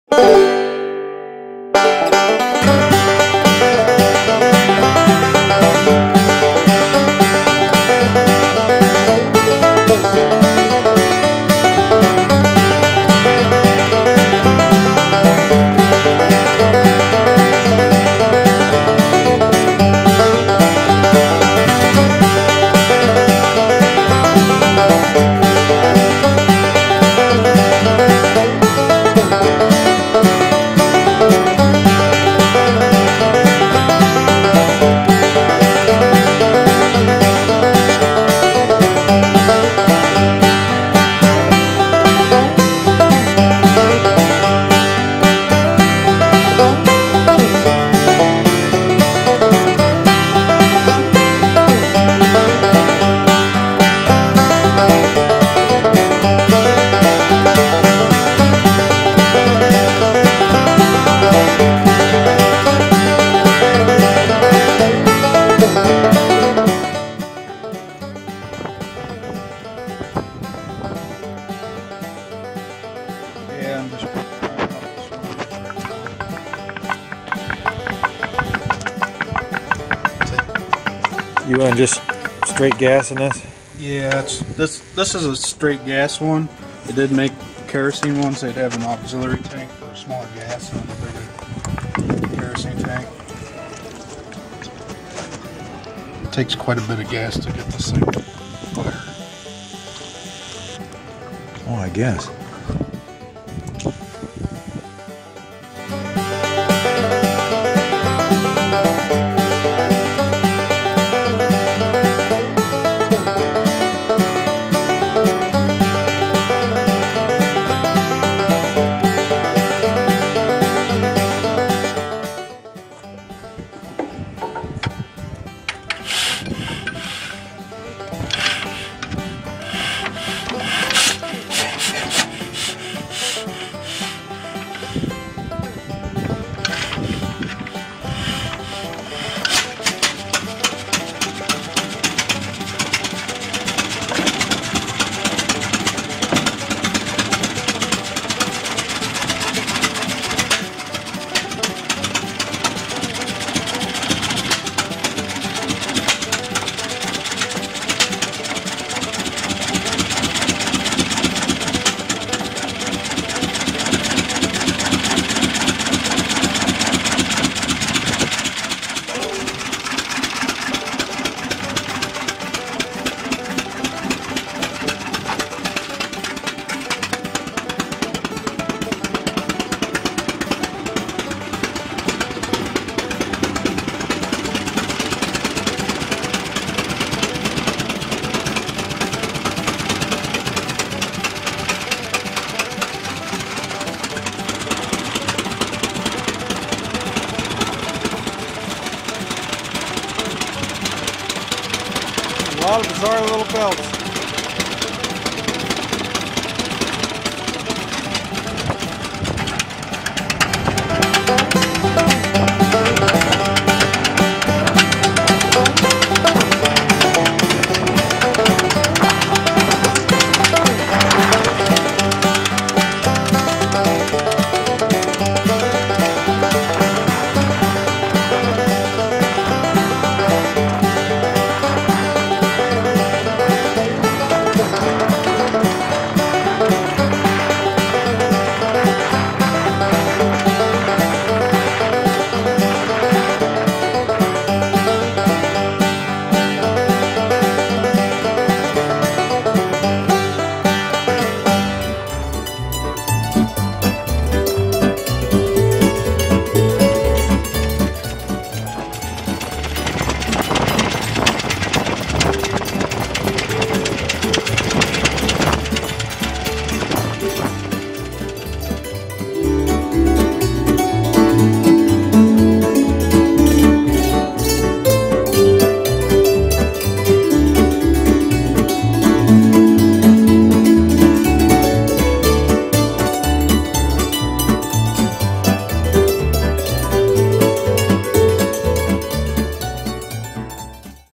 The Little Bull Tractor
The Bull Tractor.  The first tractor marketed by Massey Harris.